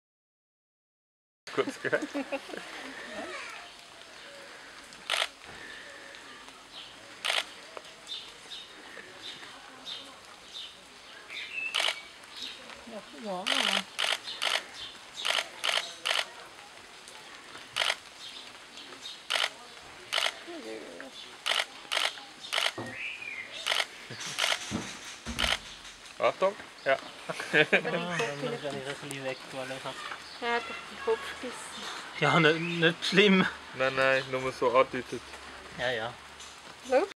Petting Mojak the lynx II
Another video of myself petting Mojak the lynx. He seems to love me, but at the end it was getting a bit dangerous! ;)